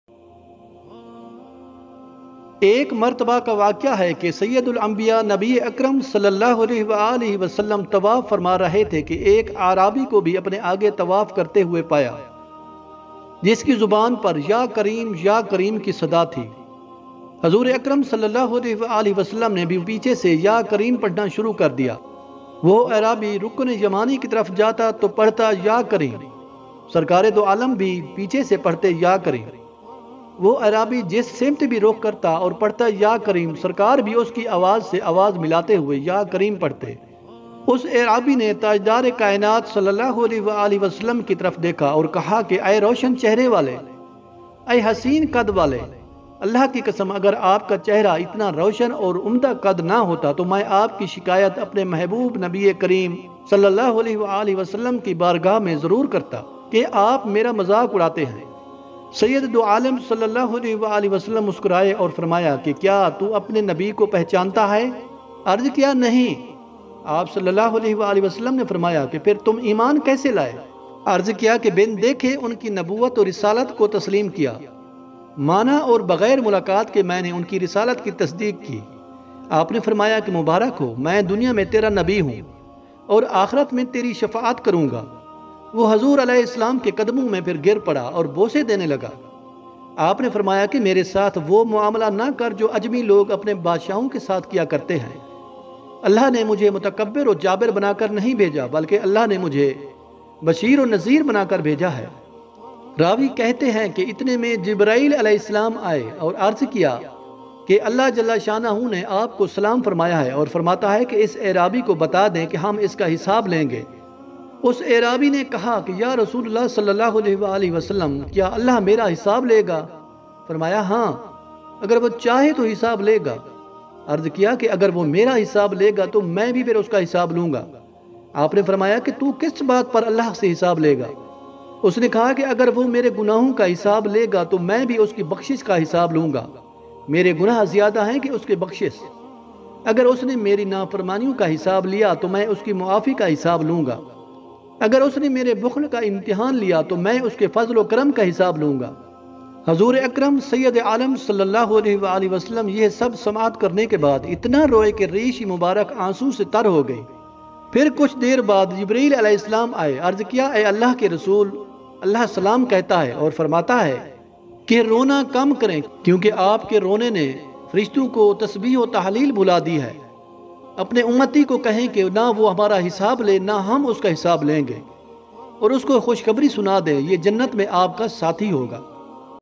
The story of Holy Prophet Muhammad ﷺ in urdu, Ek Arabi Ka Tawaf Karny Ka Waqia, Islamic stories
The story of Holy Prophet Muhammad ﷺ in urdu